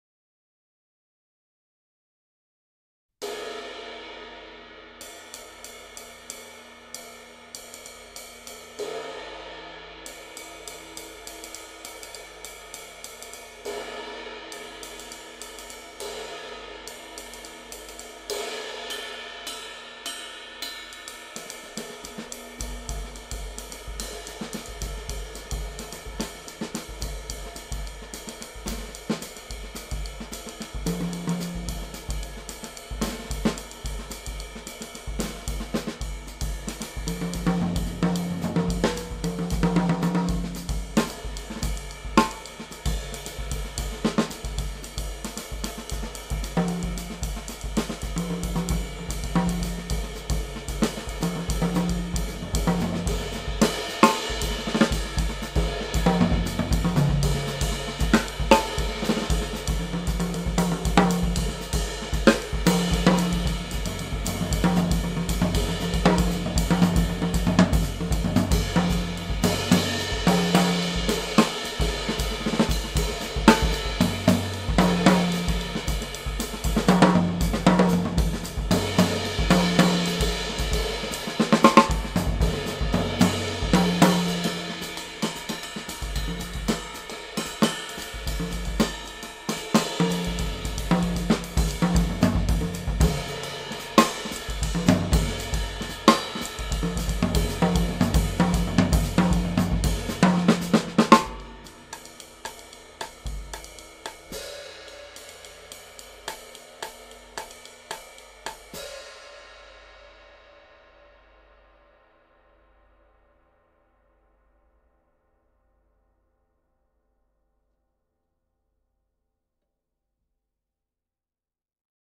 Zildjian K Constantinople 22" Medium Thin Ride High (2578g)
GEAR LIST: Yamaha PHX 16" x 20" bass drum, 8" x 10" & 9" x 12" mounted toms, and 13" x 14" floor tom; DW Super Solid 6" x 14" Maple & Cherry Edge snare; Zildjian K Constantinople 14" Hi Hats (912 & 1184g).